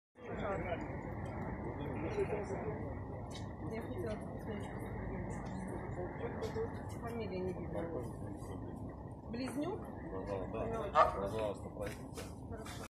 Видео задержания доверенного лица Светланы Тихановской. Как Лукашенко устраняет конкурентов.
Новости о протестах в Беларусии.